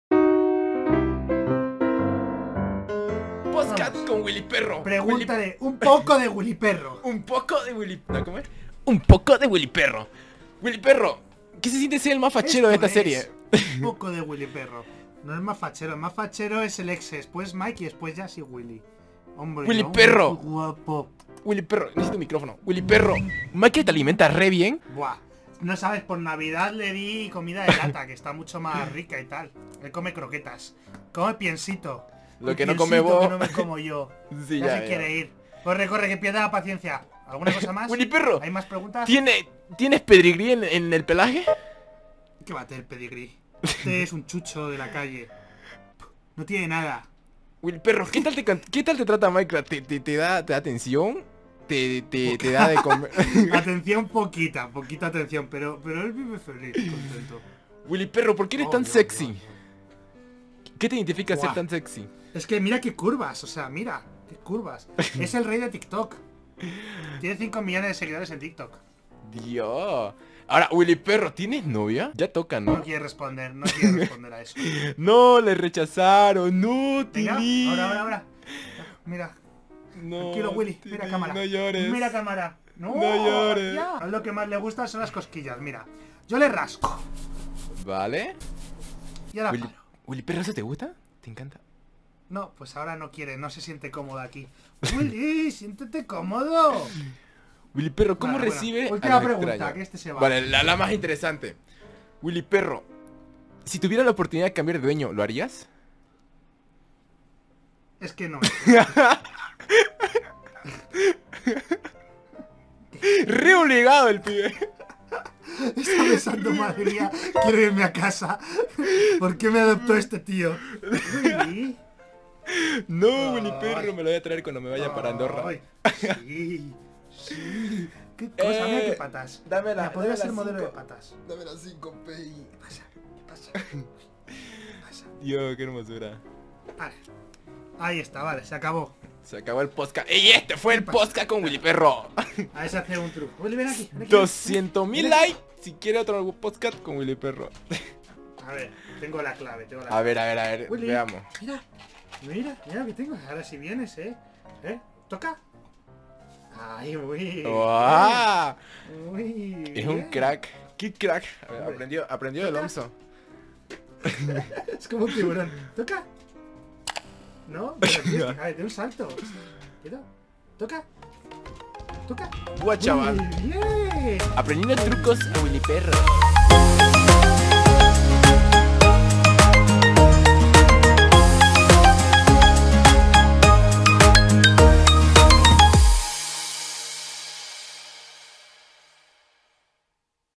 ¡PODCAST CON WILLYPERRO! 🐕‍🦺🎙 ¿WillyPerro TIENES NOVIA? 😱🦮
Hoy veremos una grabación un poco random xD que hicimos con willyperro 🥰 haciéndoles preguntas de su vida personal… 🐶🐕‍🦺

Atte: @Omzo74YT

⭐ ADMINISTRADORES ⭐